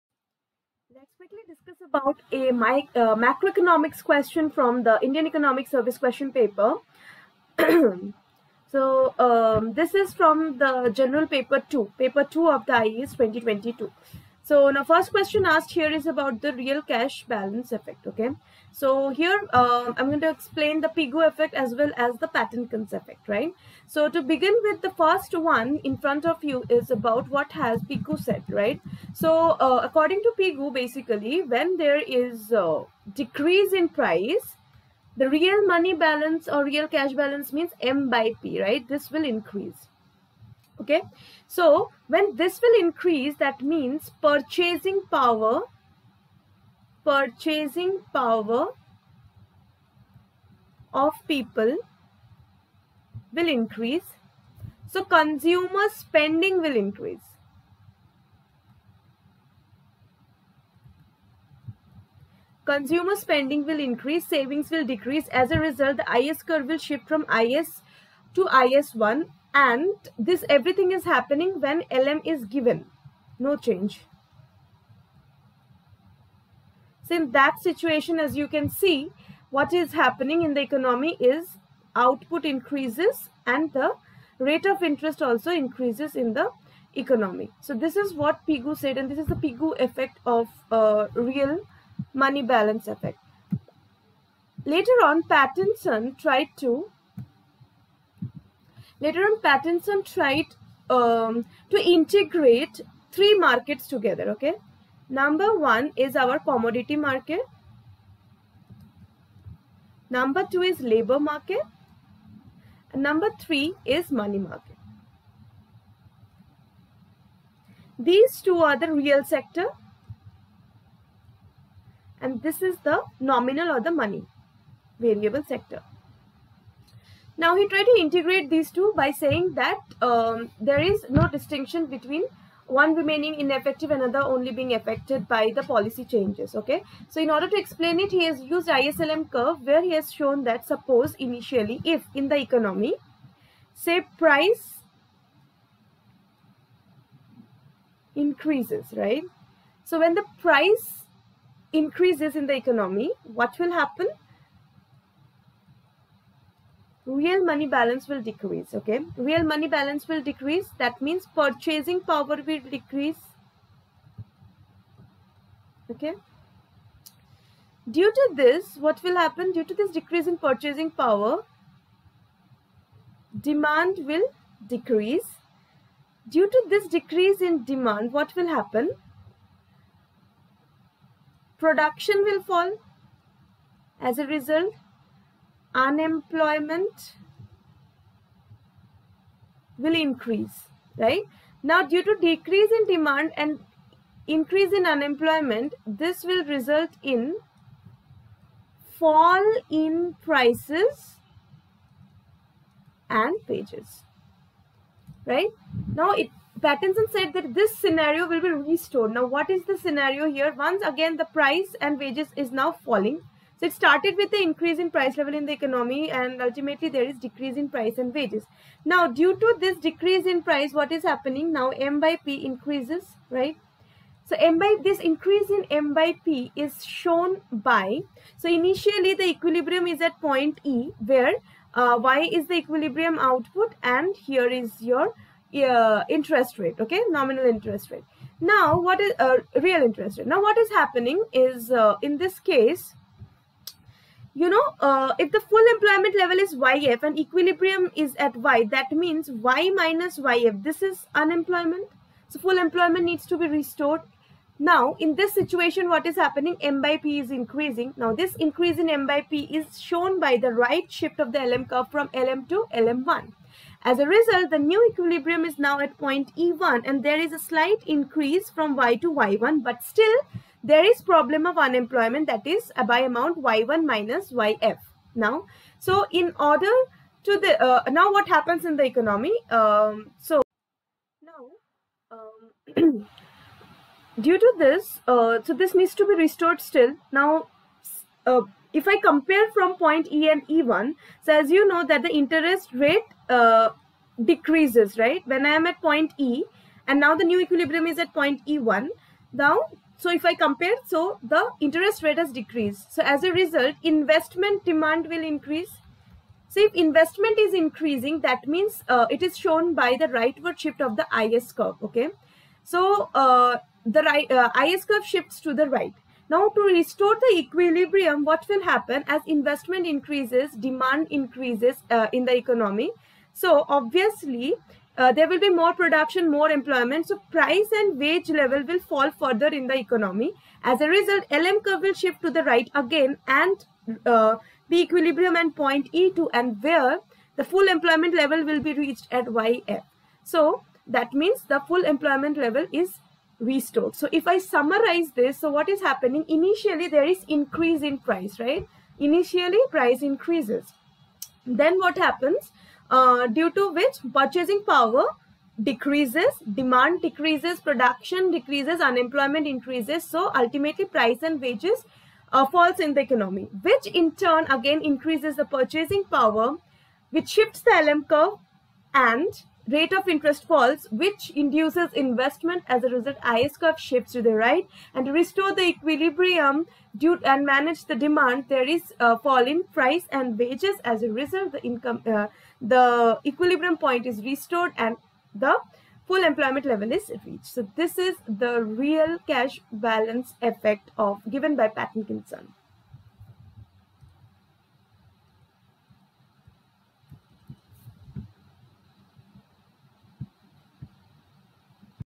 Real Cash Balance Effect_ Indian Economic Service_ General Paper 2_ Solutions
The real cash balance effect, also known as the real balance effect, is an economic concept that describes how changes in the purchasing power of cash balances can affect aggregate demand and the overall level of economic activity in an economy.

The real cash balance effect is based on the idea that people hold a portion of their wealth in the form of cash, which they can use to purchase goods and services. When the value of cash balances increases, due to a decline in prices or an increase in the nominal money supply, the real value of cash balances also increases. This means that people can purchase more goods and services with the same amount of cash, which leads to an increase in aggregate demand.

Conversely, when the value of cash balances decreases, due to an increase in prices or a decrease in the nominal money supply, the real value of cash balances also decreases. This means that people can purchase fewer goods and services with the same amount of cash, which leads to a decrease in aggregate demand.

The real cash balance effect is an important concept in macroeconomics, as it helps to explain how changes in the money supply and prices can affect the level of economic activity in an economy. When the real value of cash balances increases, people tend to spend more money, which leads to an increase in aggregate demand and economic activity. Conversely, when the real value of cash balances decreases, people tend to spend less money, which leads to a decrease in aggregate demand and economic activity.

The real cash balance effect is also related to the concept of the velocity of money, which measures the rate at which money circulates through the economy. When the real value of cash balances increases, people tend to spend more money, which increases the velocity of money and leads to a multiplier effect on aggregate demand. Conversely, when the real value of cash balances decreases, people tend to spend less money, which decreases the velocity of money and leads to a negative multiplier effect on aggregate demand.

📌WHATSAPP AT

FOR

 📚   STUDY MATERIALS